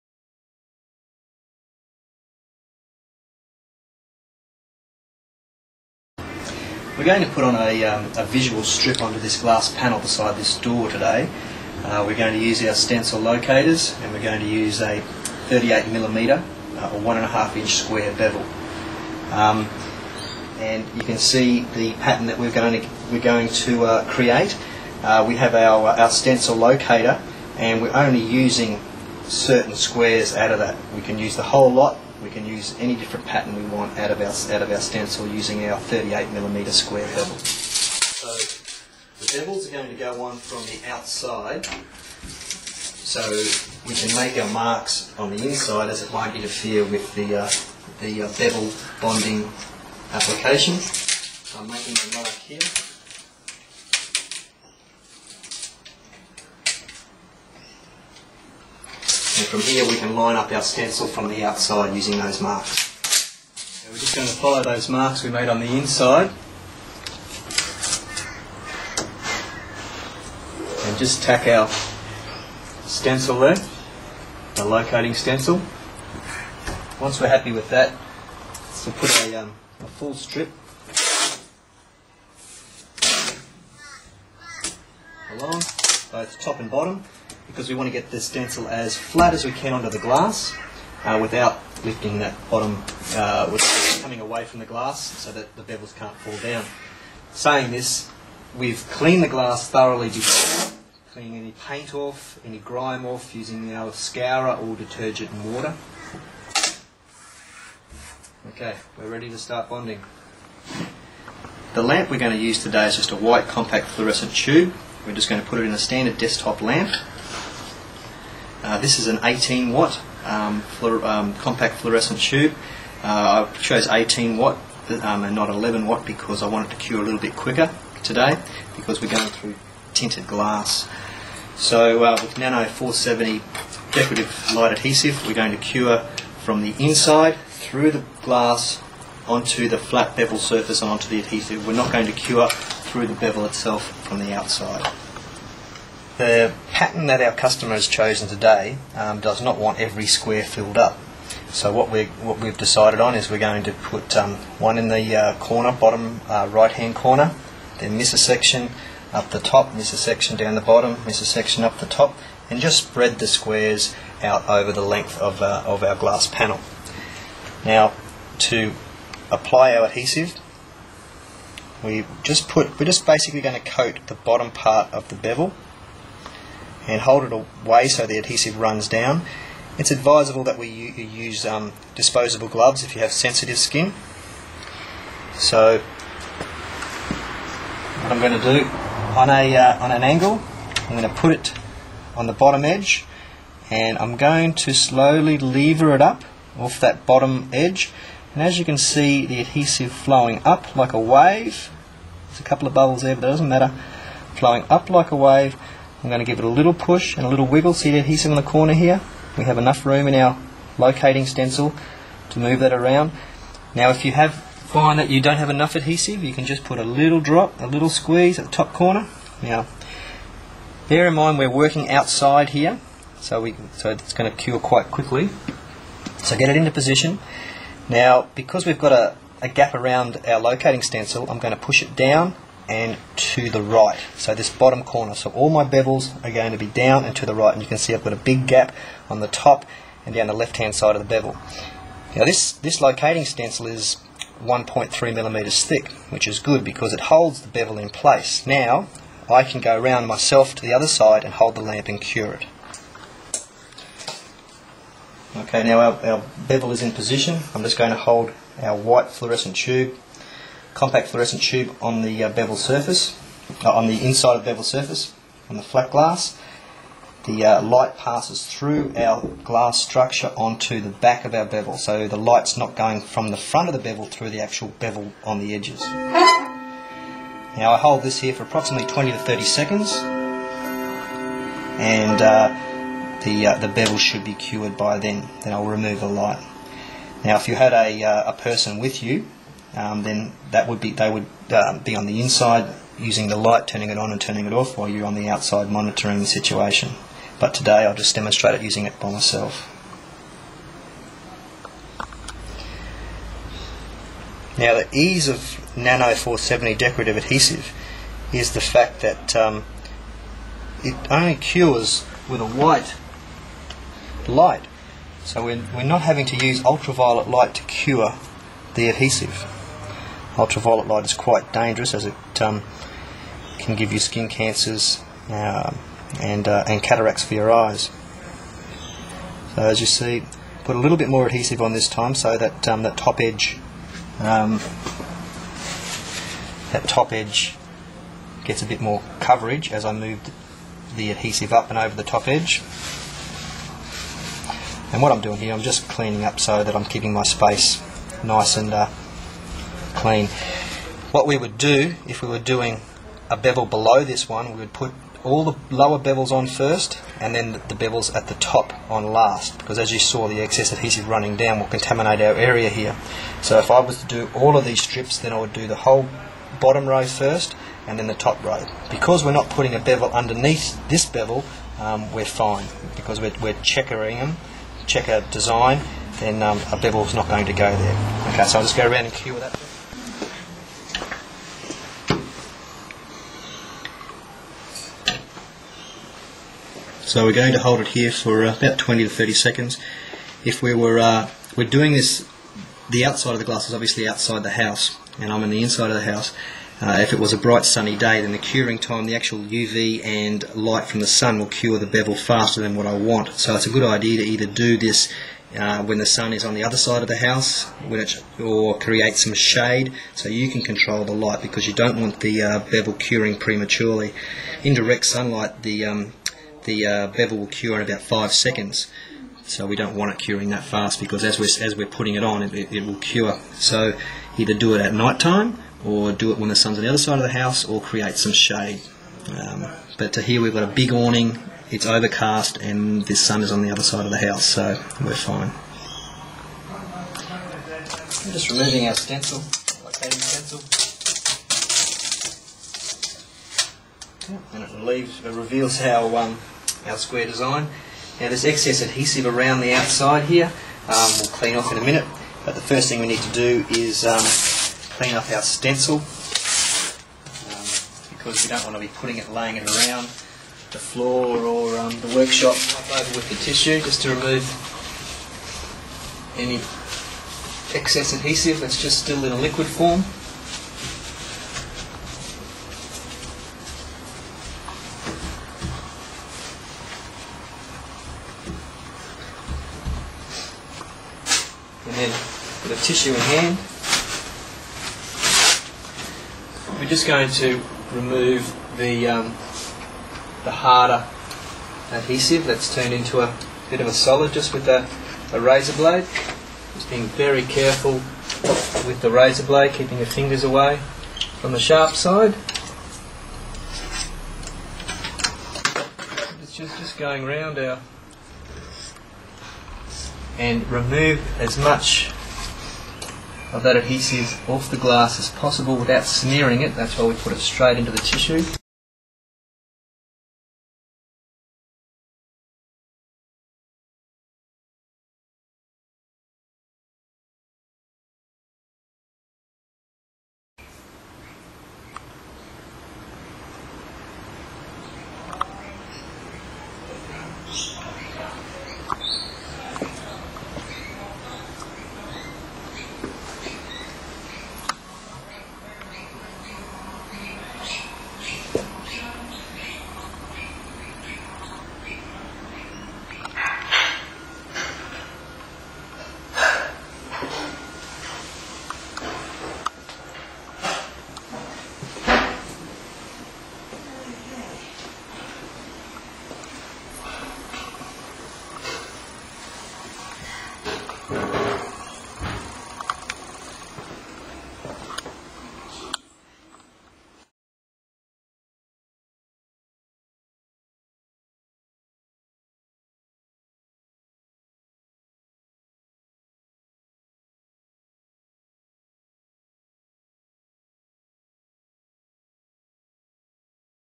Glass square bevel, glass bevels with adhesive making a visual strip
Nano470 Glass2glass visual strip on side panels . Glass bevels and clusters are easily glued / bonded onto glass. This video shows a simple square bevel bonded in a pattern using nano470 glass2glass adhesive